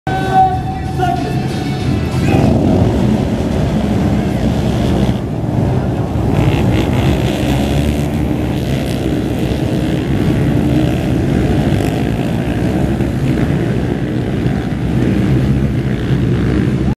MX IXCR Bakers Camp Hunter's Start
Hunter Harris get the hole shot for the Super Mini Youth Quads